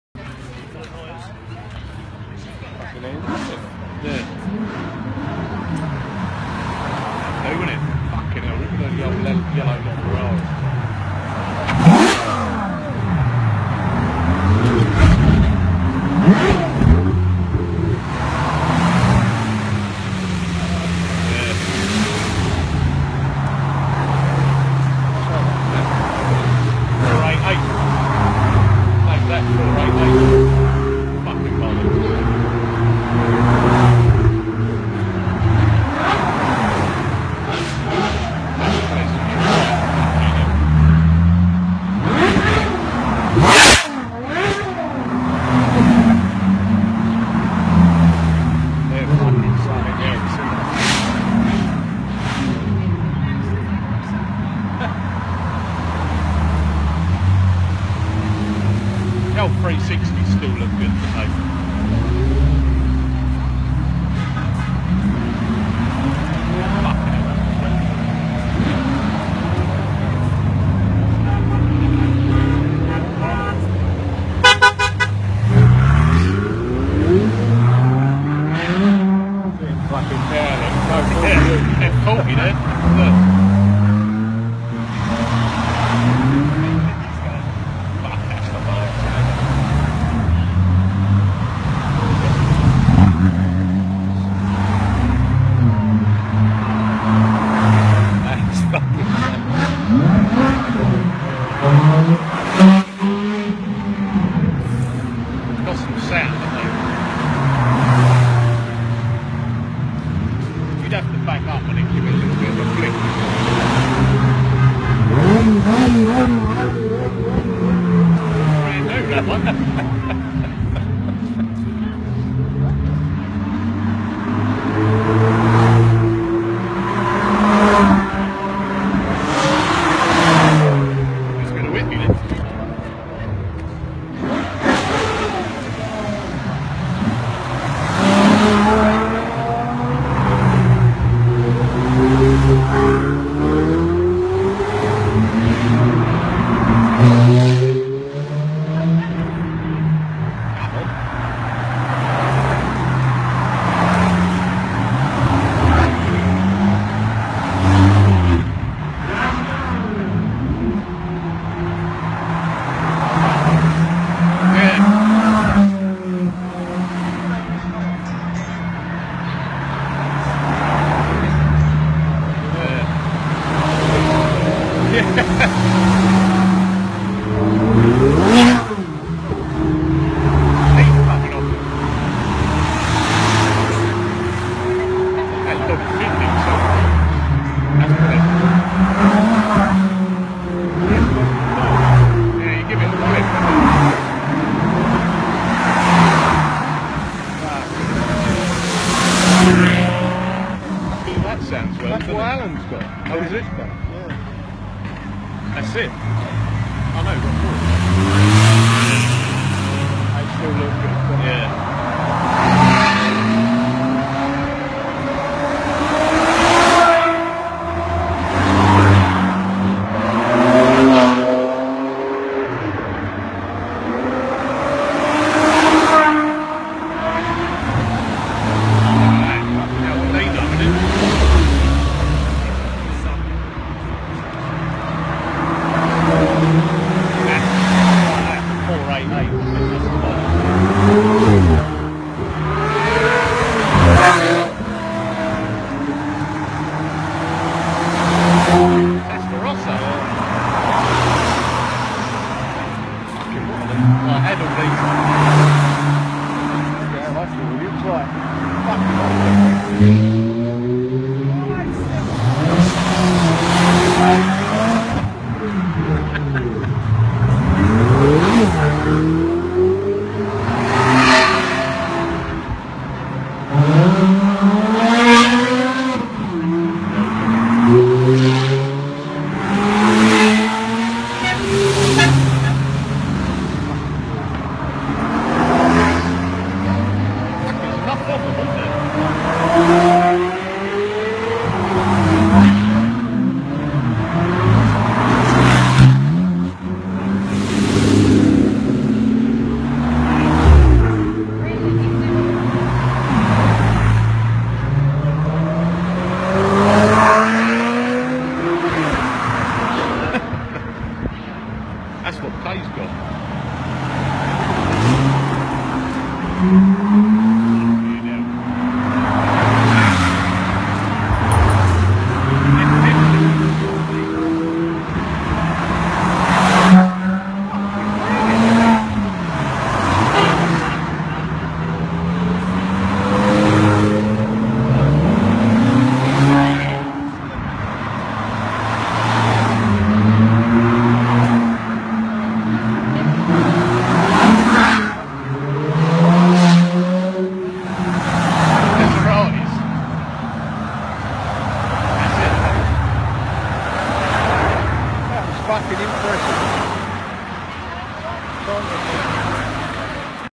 70 Years Of Ferrari Parade Through Essex Stock, Billericay Offset Detailing Essex
Certified Kamikaze Collection Detailer & Auto Finesse Approved.

An incredible array of Ferrari's new and old cruising through Essex a while back. Filmed outside the Hoop pub with a few beers - what a sight!